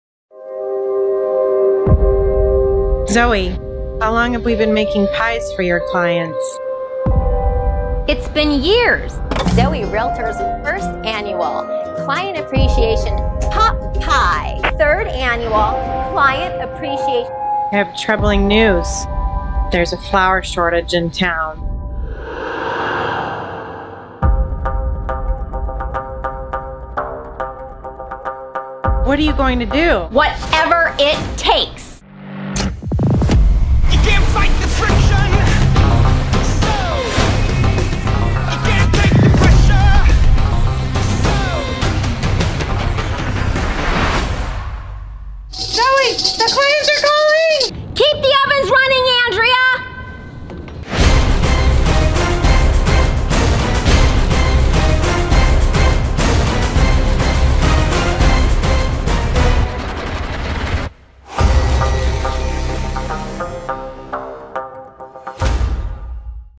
ZOEE Realtor MISSION ImPIEssible CLIENT APPRECIATION POP PIE EVENT
ZOEE Realtor's Annual Client Appreciation THANKSGIVING POP PIE EVENT! In gratitude for your business and continued referrals, I want to buy your Thanksgiving Pie!